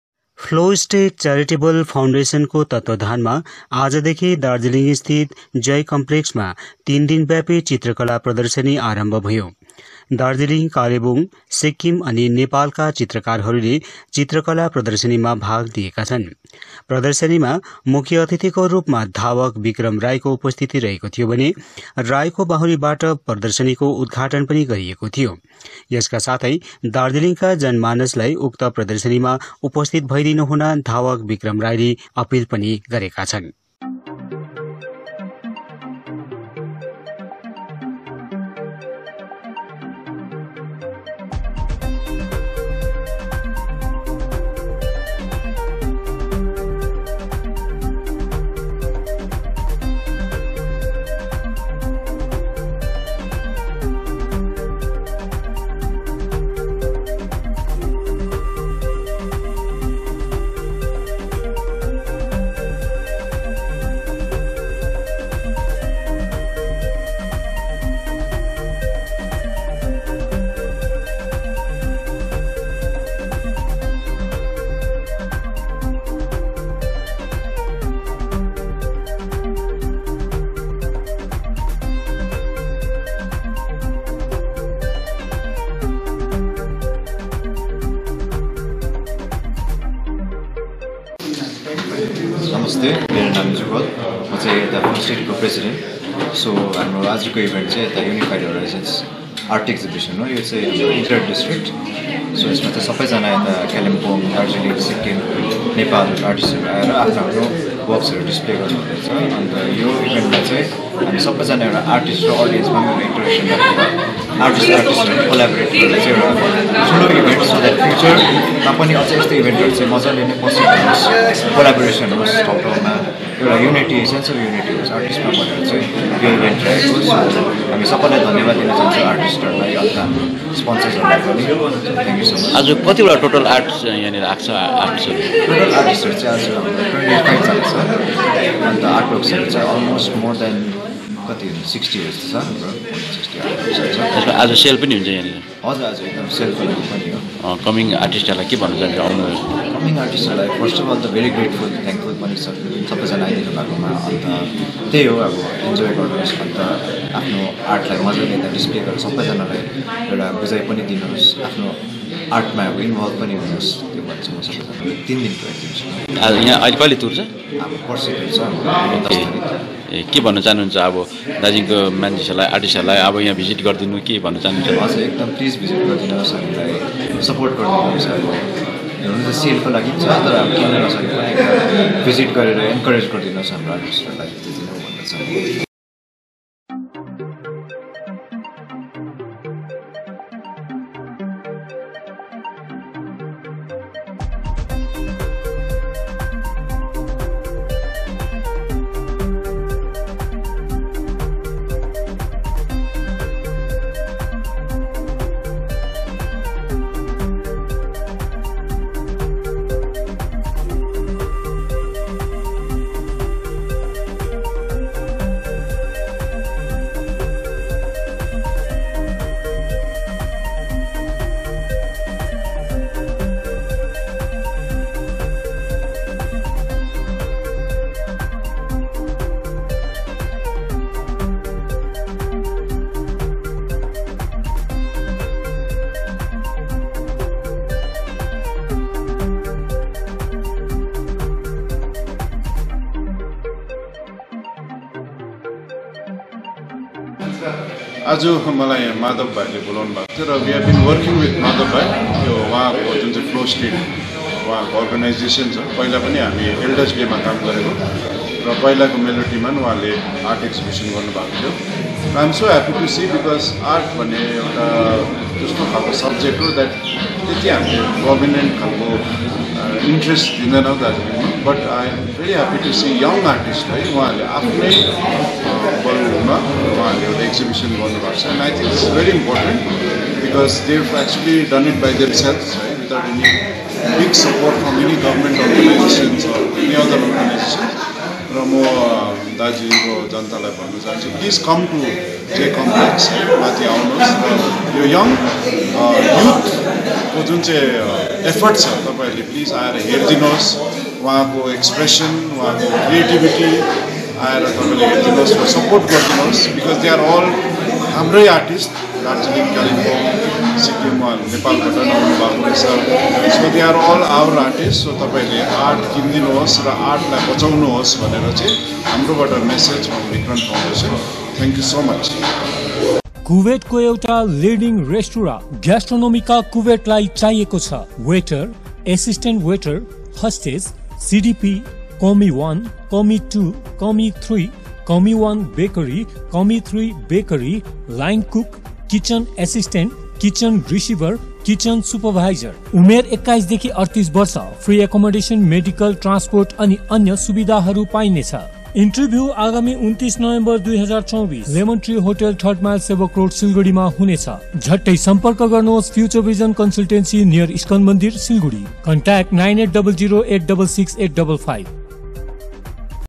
आज देखि दार्जीलिङमा तीन दिन व्यापी चित्रकला प्रदर्शनीको शुरुआत
फ्लो स्टेट च्यारिटेबल फाउन्डेसनको तत्वावधानमा आज देखि दार्जीलिङमा  तीन दिन व्यापी चित्रकला प्रदर्शनीको शुरुआत